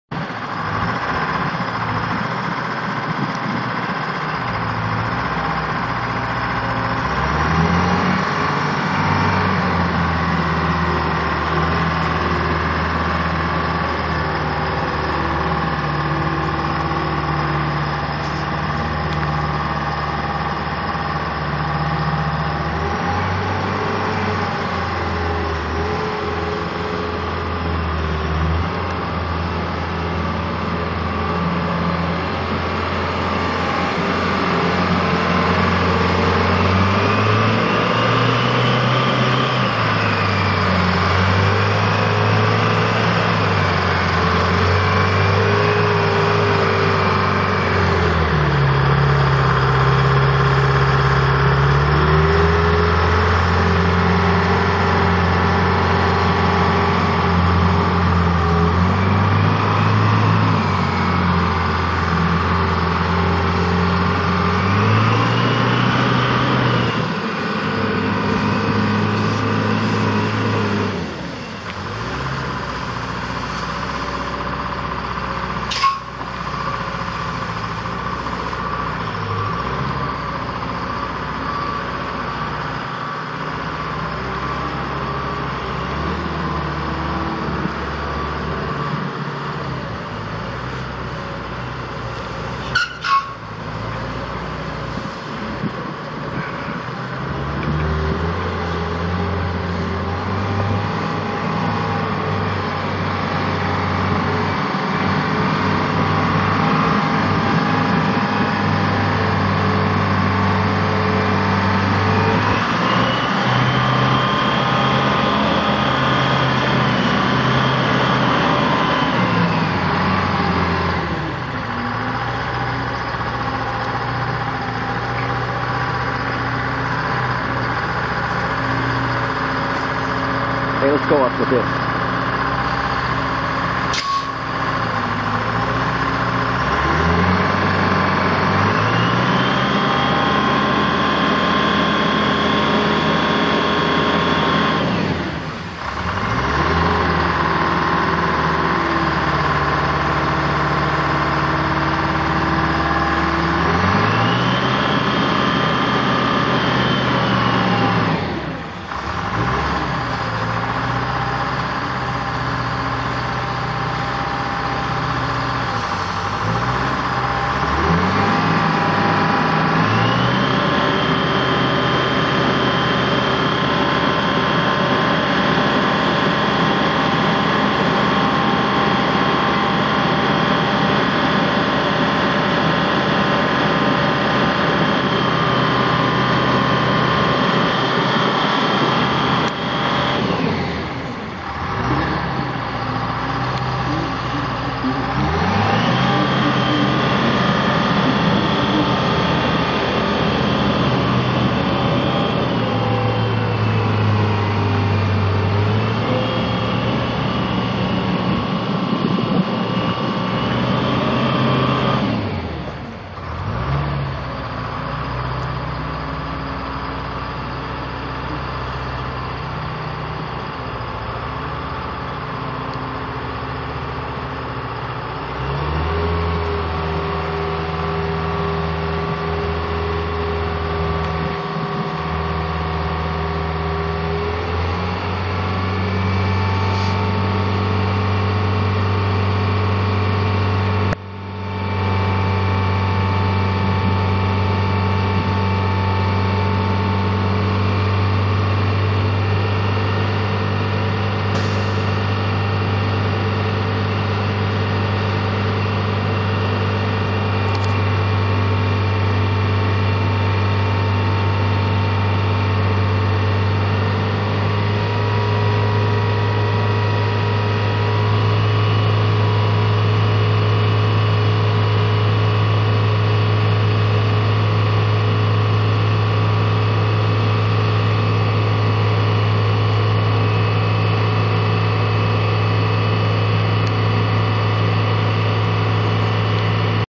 1979 Grove RT518 crane for sale at auction | bidding closes January 17, 2019
1979 Grove RT518 crane
260 hours on meter
Detroit Diesel 4-53N 212 C.I.D. four cylinder turbo diesel engine
115 HP at 2,600 RPM
Six speed manual transmission
Single axle
Four wheel drive
All wheel steer
Suspension seat
Stick crane controls
Telescopic boom
25' boom length
Three boom sections
15,560 lbs max lift capacity
60' max tip height
Winch
In-cab boom controls
(3) sheaves
18 ton capacity

(4) stabilizers
Hydraulic brakes
121" wheelbase
16.00x25 tires
GVWR 44,871 lbs
Boom extends, but will not retract
Front outriggers inoperable
Serial 42830
It's located in Texarkana, TX, and we're selling it to the highest bidder regardless of price.